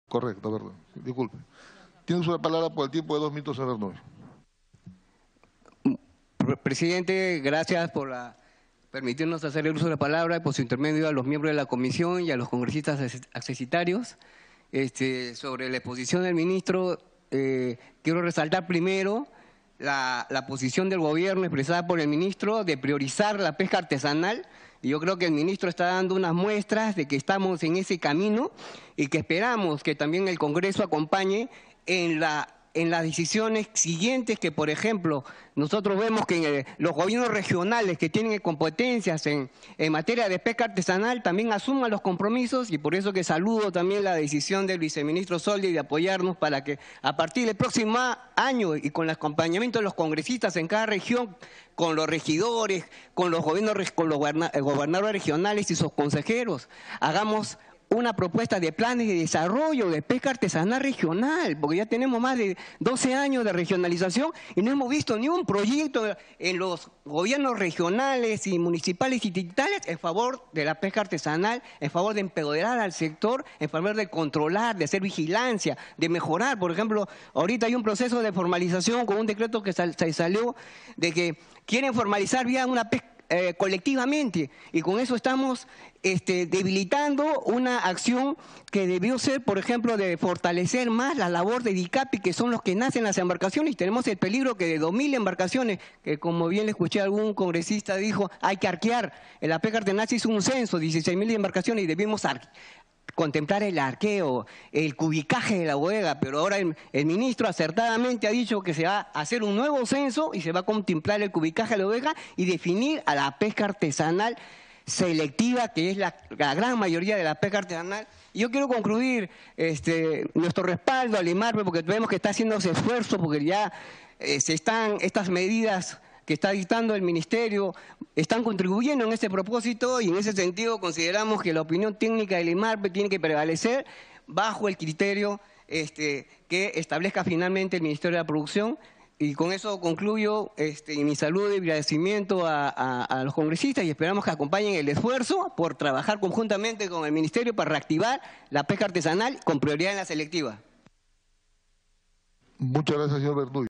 Priorización de la pesca artesanal por el Ministro Giuffra respalda ANEPAP
Presidente de ANEPAP José Luis Bernuy respaldo decisión del Gobierno y del Ministro de la Producción Bruno Giuffra de priorizar el desarrollo de la pesca artesanal selectiva, y acciones que PRODUCE comenzó a realizar, durante su intervención en la sesión ordinaria de la Comisión de Producción del Congreso de la República que preside Martín Mantilla del día 13 de Diciembre del 2016, en la que el Ministro informo sobre la pesca de la anchoveta. Fuente: Congreso de la República del Perú.